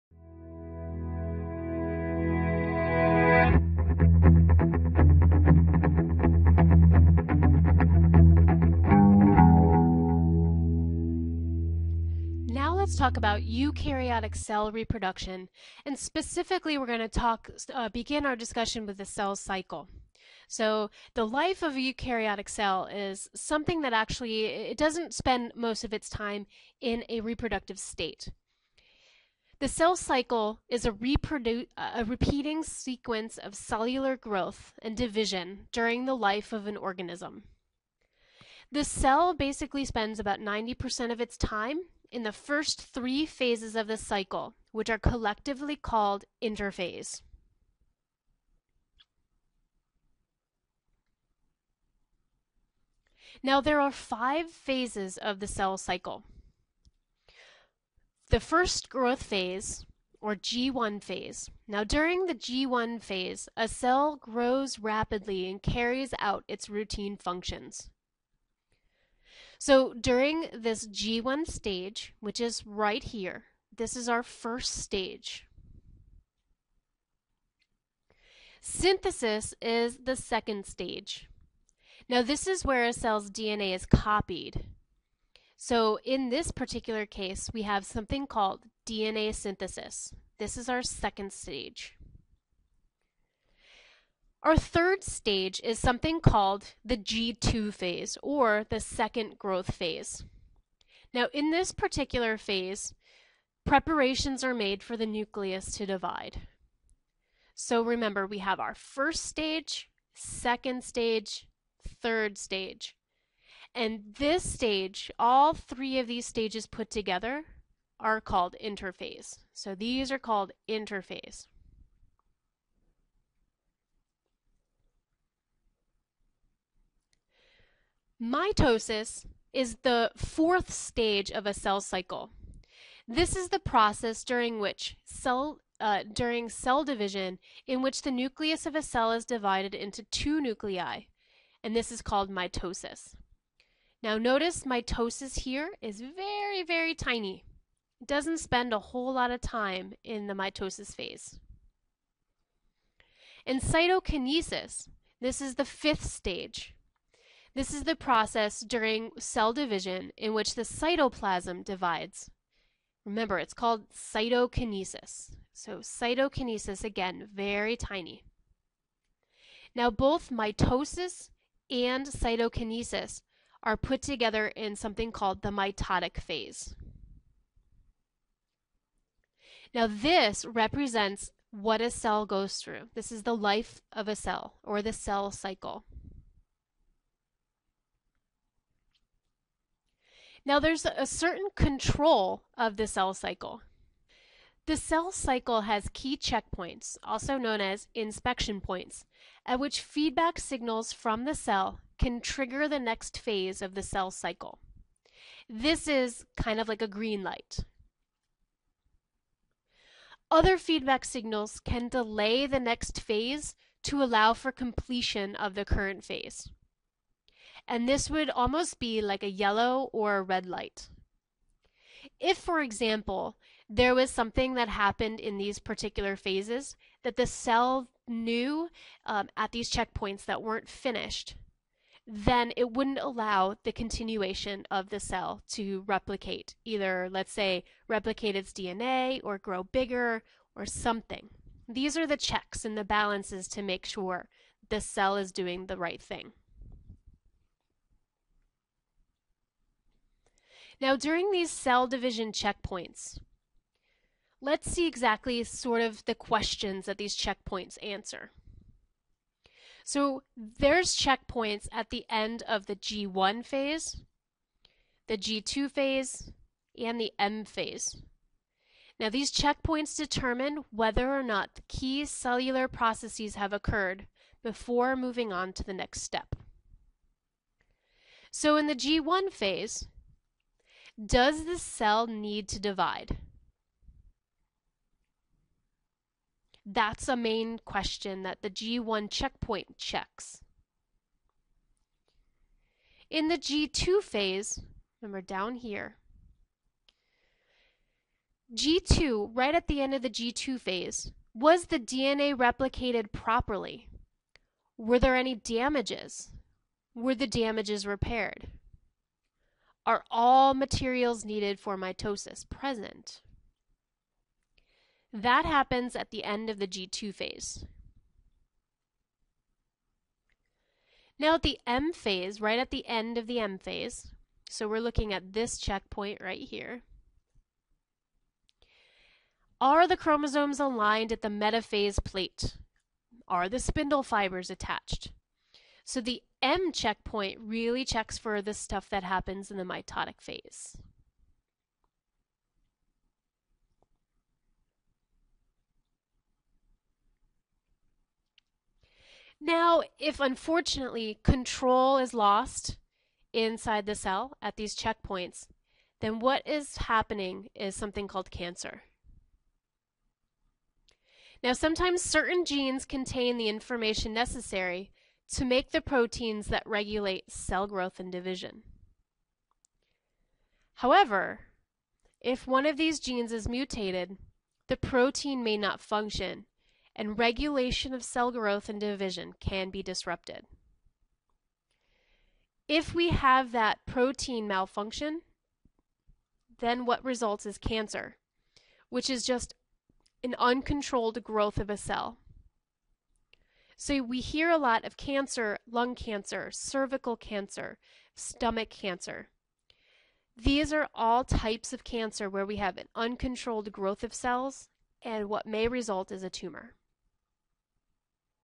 PABIO Unit 5: Eukaryotes - Cell Cycle
Describe the main events of the cell cycle. Explain how mutation and uncontrolled cell growth leads to cancer.